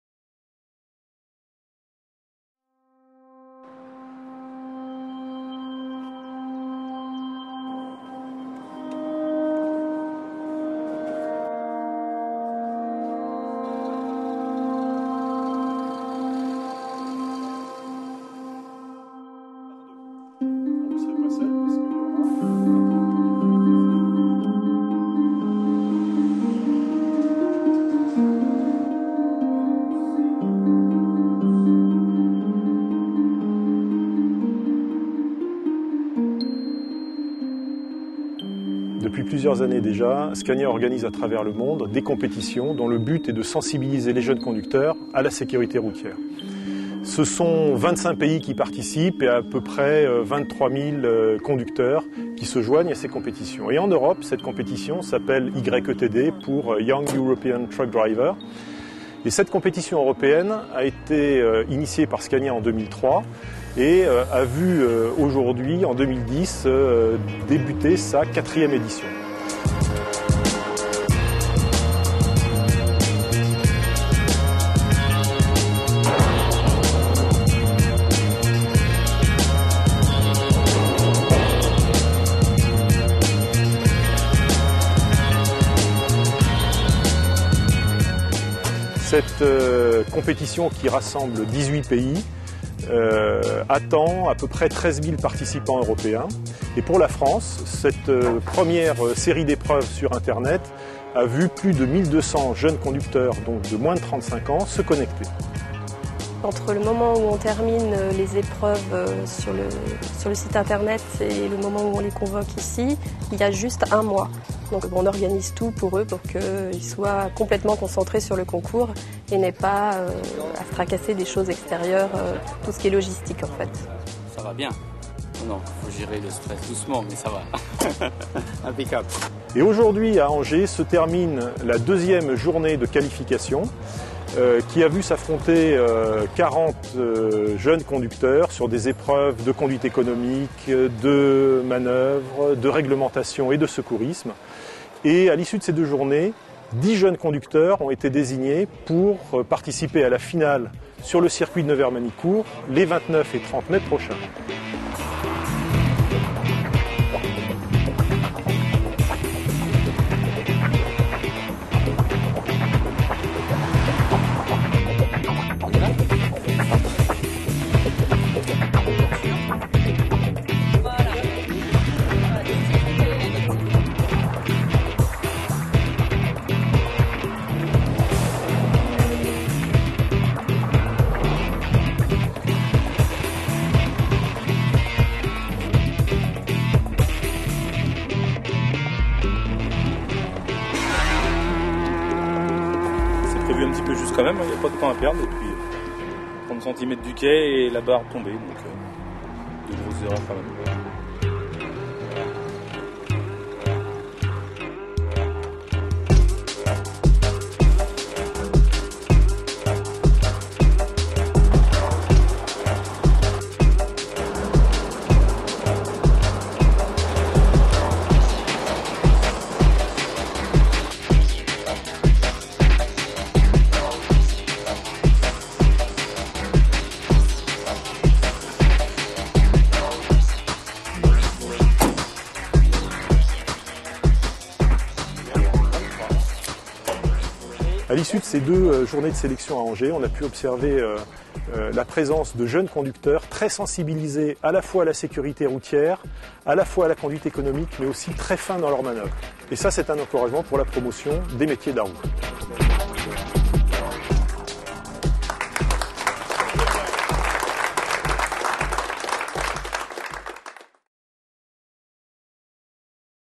Premières qualifications françaises YETD 2010
Les qualifications à la finale de l'édition française du concours du " Jeune Conducteur Européen" de l'année 2010 se tenaient les 29 et 30 avril au siège de Scania France à Angers.

Ces qualifications reposaient sur une série d'épreuve comprenant notamment des tests pratiques de manoeuvres de précision et de conduite économique, ainsi que des épreuves théoriques relatives à la réglementation et à la sécurité.

Les 10 candidats sélectionnés se retrouveront les 29 et 30 mai prochain sur le circuit de Nevers Magny Cours pour la grande finale française YETD 2010 à l'occasion du Grand Prix Camion.

Les 10 conducteurs qualifiés sont :

Ludovic CESCHIN St-Jorioz (74)
Sylvain CHASSINE Epieds-en-Beauce (45)
Frédéric DIVAY St Georges-de-Chesné (35)
Jérôme FICHET St-Romain-de-Surieu (38)
Vincent GRUSELLE Ludres (54)
Stephen LACOMBE Lay (42)
Thomas LACOMBE Lay (42)
Frédéric MAGIER Chabris (36)
Thomas PRIN Verquières (13)
Matthieu SEBILLET Les Essarts (85)

Reportage Truckeditions Vincent Mahe
Production exécutive Godelouprod